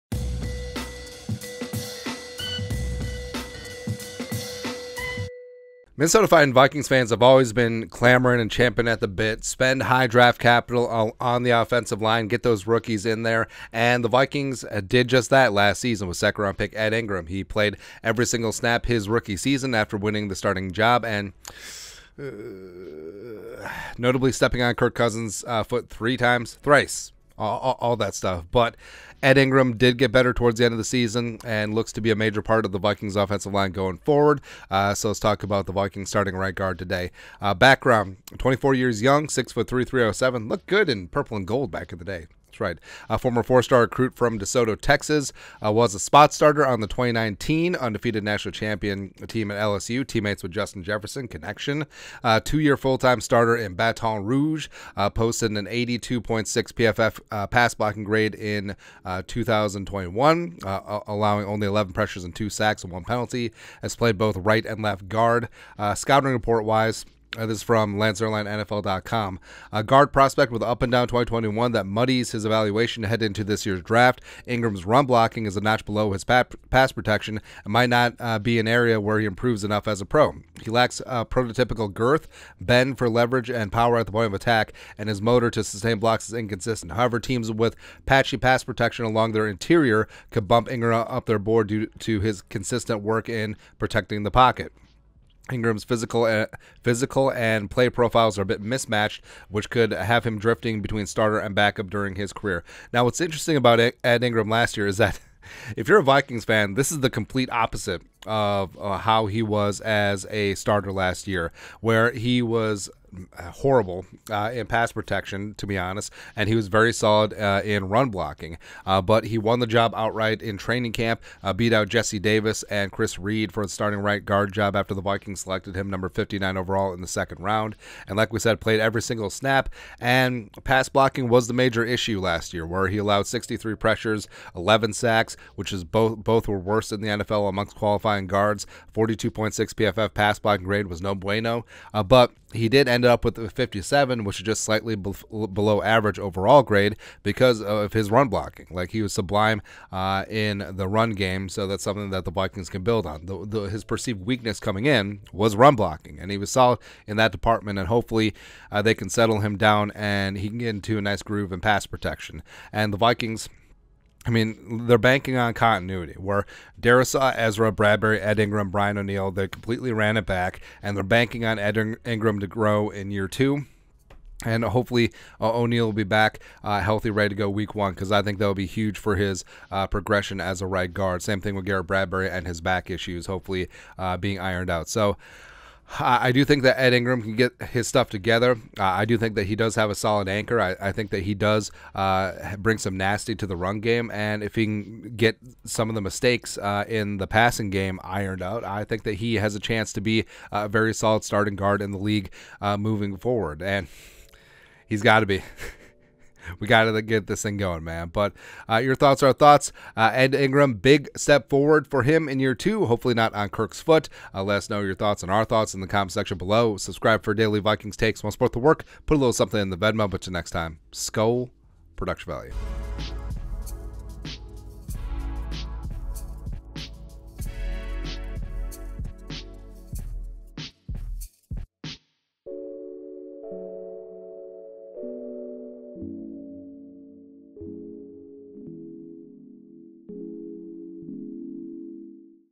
Ed Ingram: Big Step Forward in 2023? | Minnesota Vikings Training Camp Profiles
The Minnesota Vikings are banking on a major step forward for second year guard Ed Ingram.